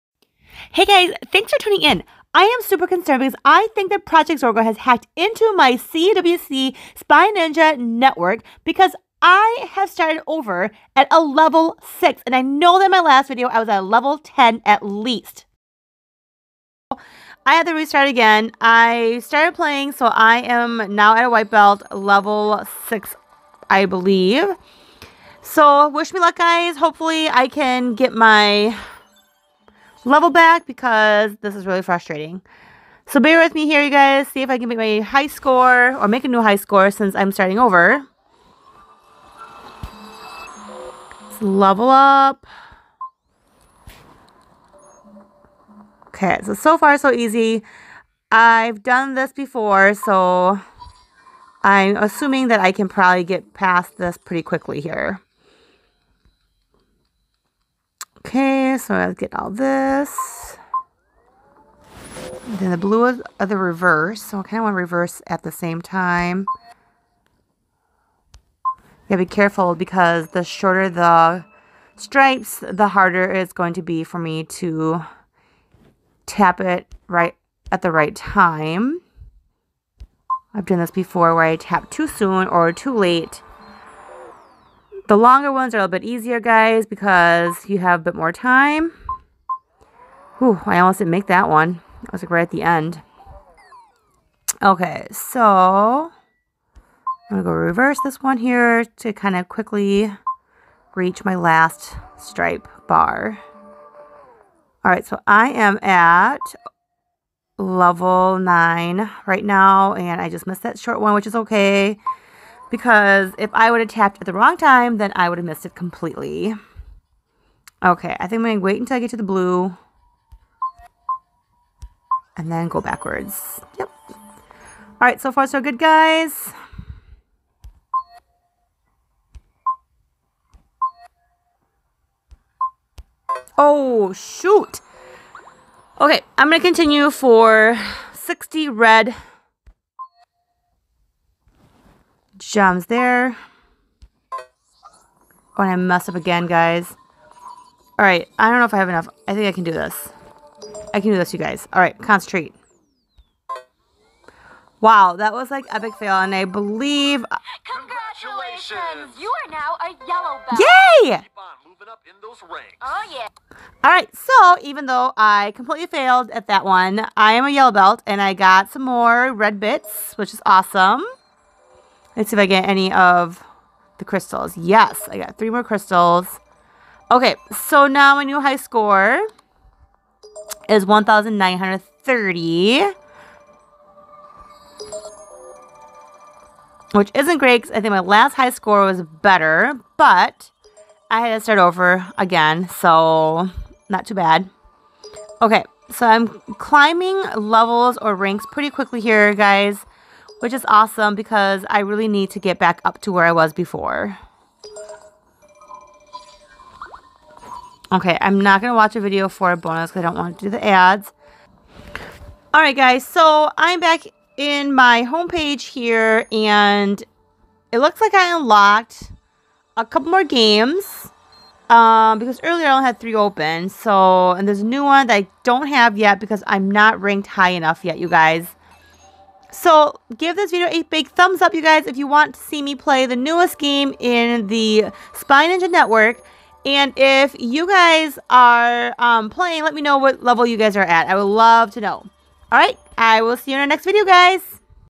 Project Zorgo Hacked into Our CWC Spy Ninja Network App!
Hey fellow Spy Ninja TEAM! I think Project Zorgo has hacked into our account on the CWC Spy Ninja Network. When we logged in today, our high scores were logged out and we were locked out of the other training room. We had to start over on LEVEL 1 as a white belt and had to start training int he Destroy the Virus room. Watch out spy ninjas!

Let us know your high score! The more we log in to the CWC Spy Ninja Network, the more we can help Chad and Vy. We can't let their organization keep growing.

Join our TEAM and get updates. It's FREE! Just click the link below 🌟🌟